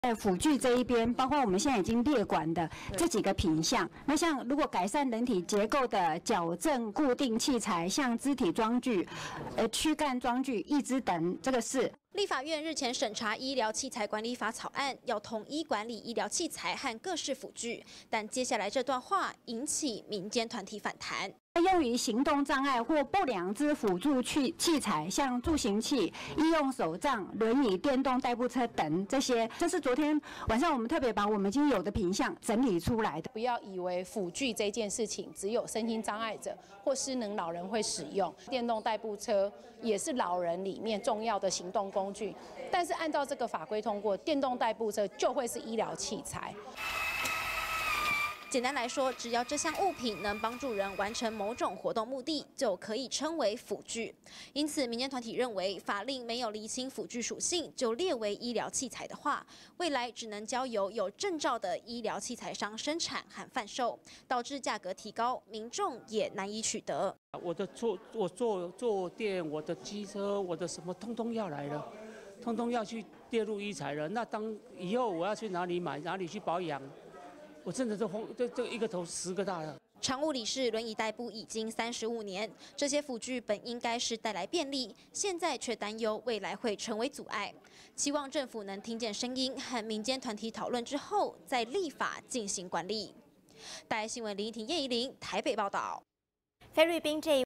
六大項輔具納入醫材
未來可能老人家常用的輪椅、助行器等輔具，都將成為醫療器材，立法院社環委員會日前審查，《醫療器材管理法》草案，要統一管理醫療器材，同時把輔具也納入管理範圍。而民間團體有不同的看法，表示很多輔具只是幫助生活更便利的器具，如果全部變成醫療器材，價格變貴，甚至難以取得。而衛福部也提出聲明，只有機械式、動力式輪椅、代步車，和醫療用拐杖會列入管理，一般民眾用來協助步行的雨傘杖、登山手杖，或用於菜市場購物之輔助推車，不列管。